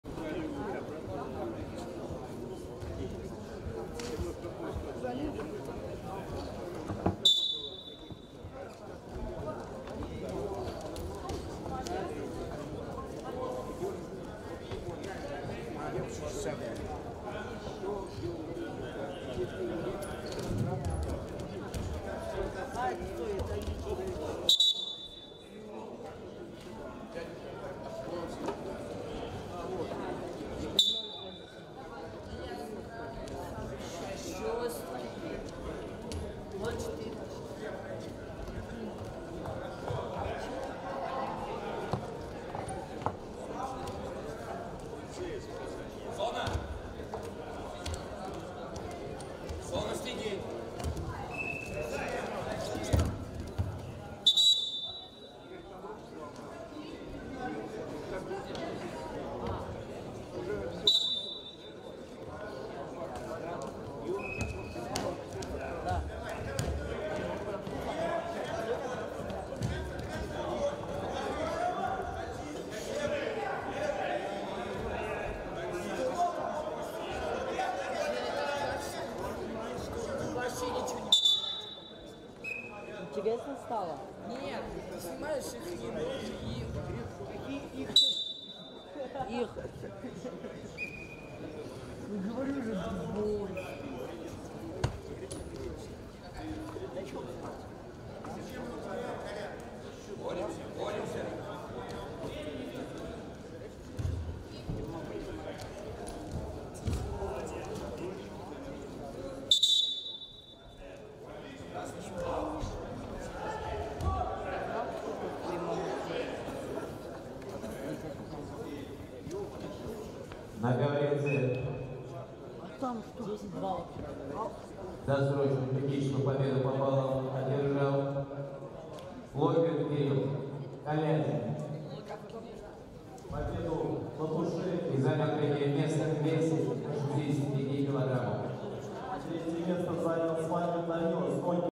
Буркут Николай ( г.Красноармейск ) - Логвин Кирилл ( г.Калязин )
VI Традиционный турнир по греко-римской борьбе на призы МС СССР В.А.Родина среди юношей 2003-04гг.р. Городской округ Мытищи.